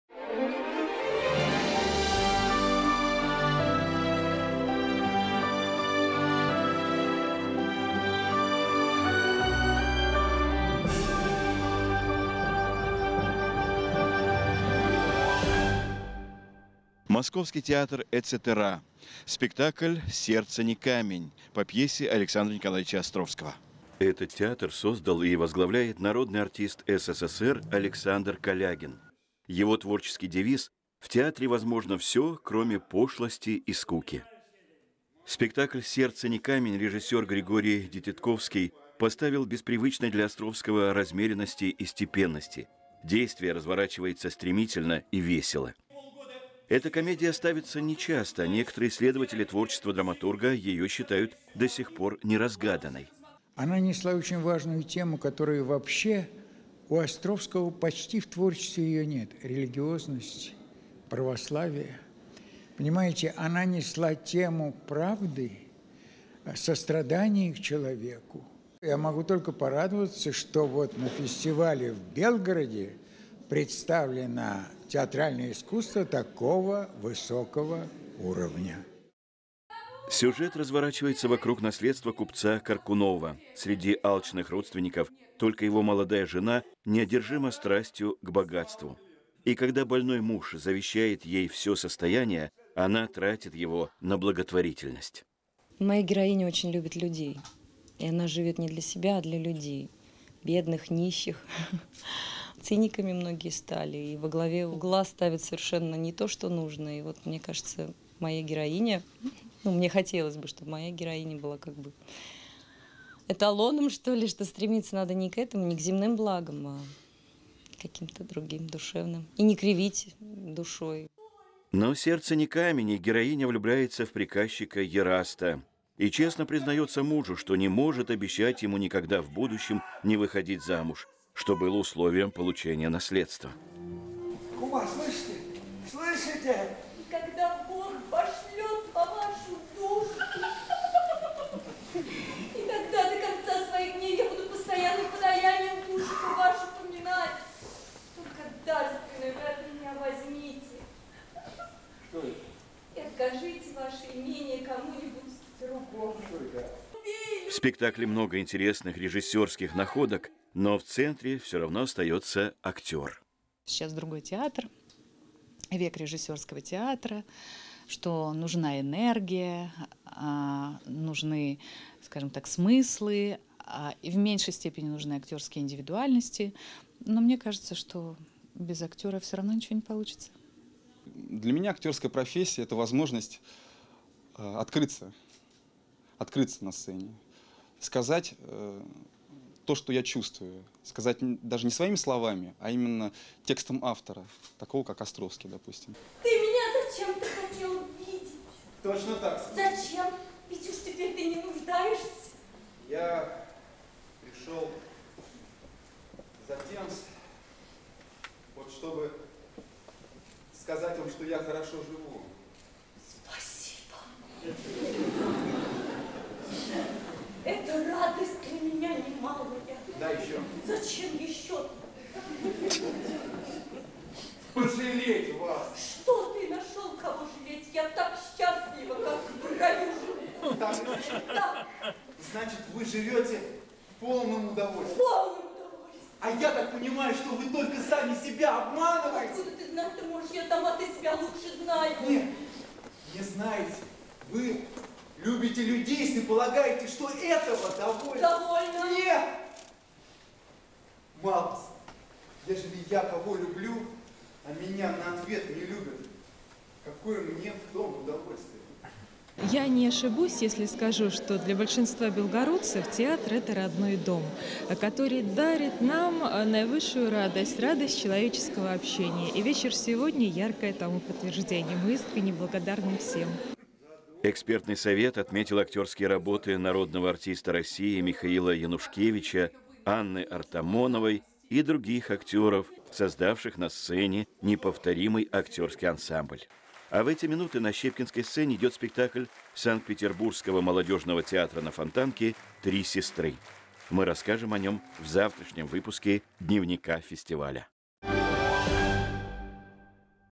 «Актеры России – Михаилу Щепкину» 2016 (часть 2)
«Мир Белогорья» представляет главное культурное событие года – юбилейный Х Всероссийский фестиваль «Актеры России – Михаилу Щепкину». Участниками этого масштабного мероприятия стали более десяти театральных коллективов со всей страны. Свои спектакли представили труппы известных театров Москвы, Санкт-Петербурга, Севастополя, Нижнего Новгорода, а также Старого Оскола и Губкина.

Вашему вниманию – видеодневник фестиваля, который подготовил для вас «Мир Белогорья». Во второй его части – мнения театральных критиков,  неравнодушных к театру зрителей и самих актеров о спектакле А. Островского «Сердце не камень».